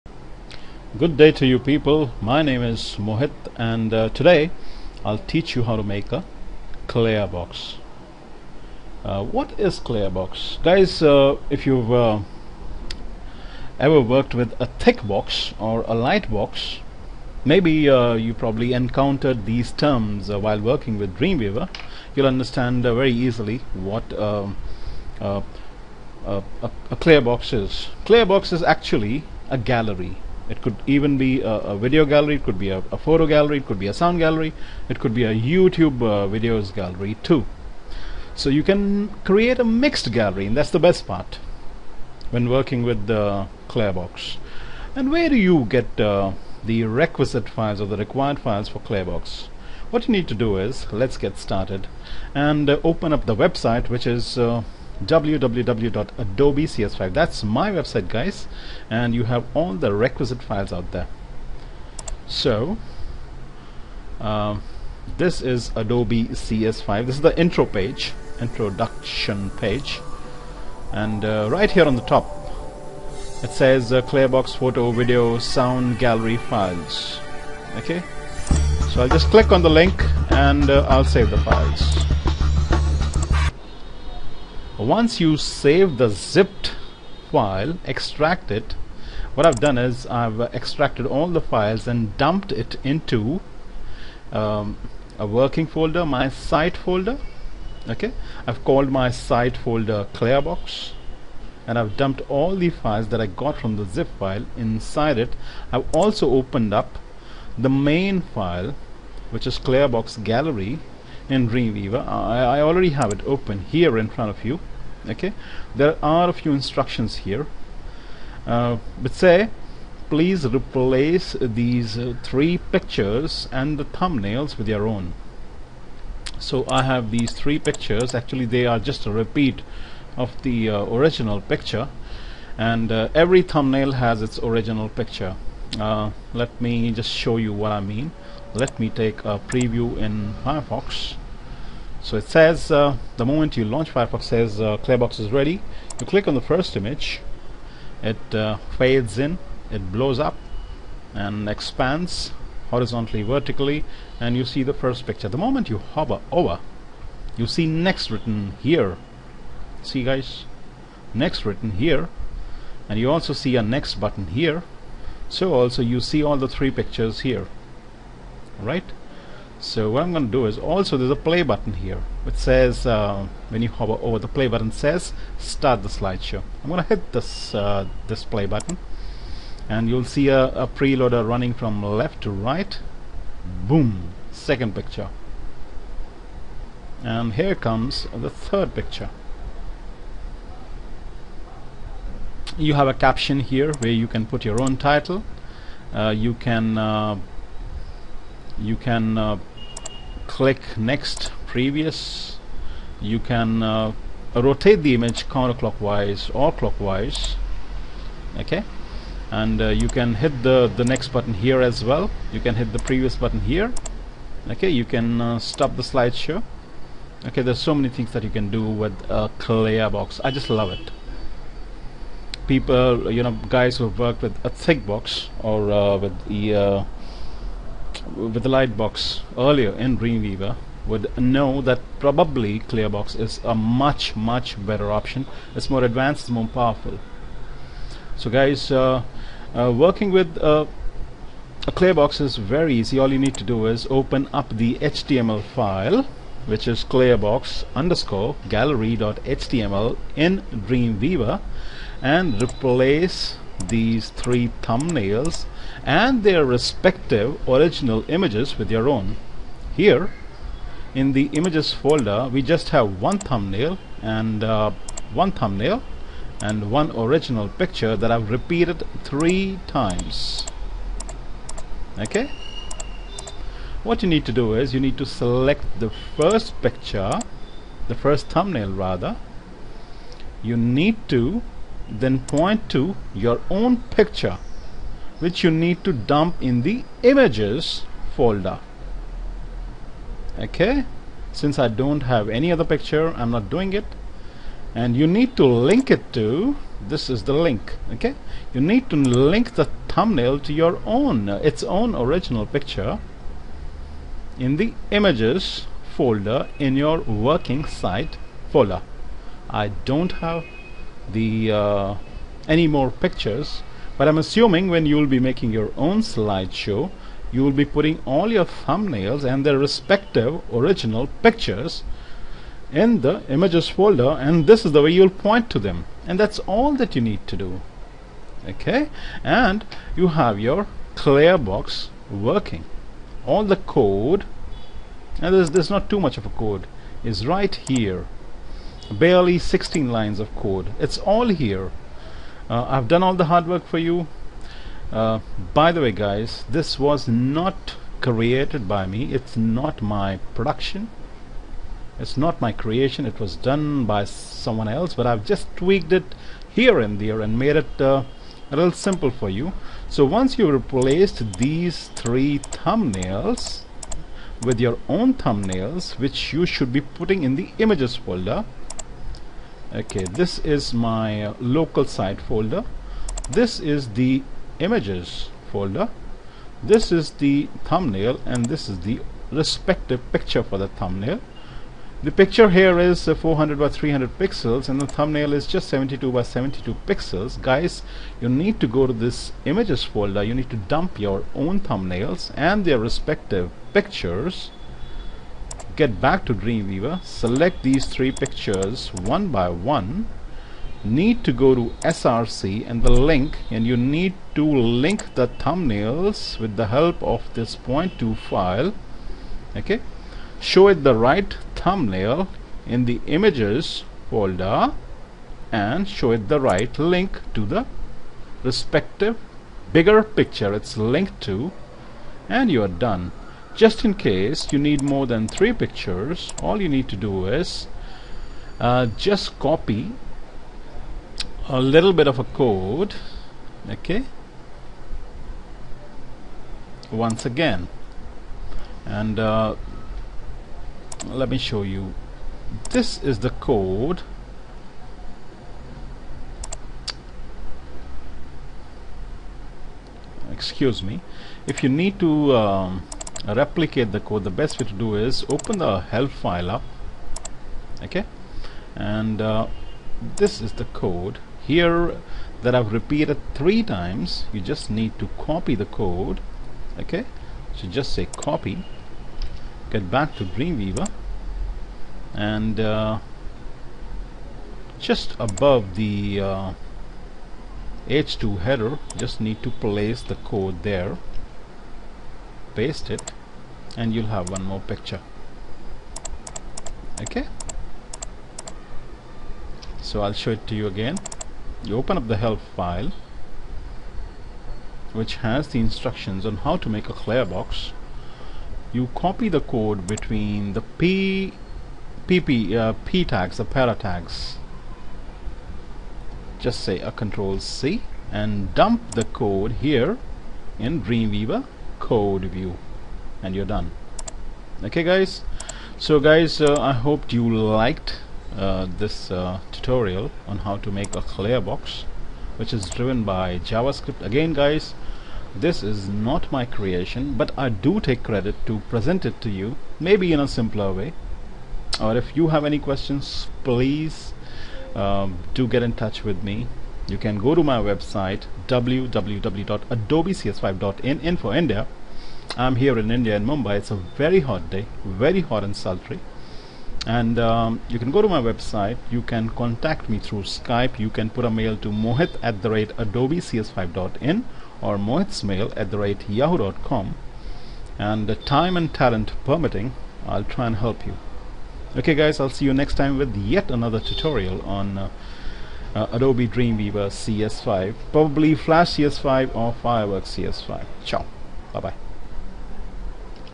Dreamweaver CS5 Tutorial: Clearbox Gallery (Make a photo or video gallery !!!)
Here I have made a photo gallery, feel free to make a video or even a sound gallery ..it will accept  media in various formats..that's clearbox for you. If you need to see  the pictures adjacent to each other you can drop the thumbnails in a Table and hence get the right grid to display your thumbnails in which case do get rid of the paragraph tags in the code view !

Mohit Manuja

Trainer for adobe products Dreamweaver , flash and fireworks cs5 at Mumbai India.

web developer

Training Dreamweaver ,flash cs5  and actionscript 3.0 online is my forte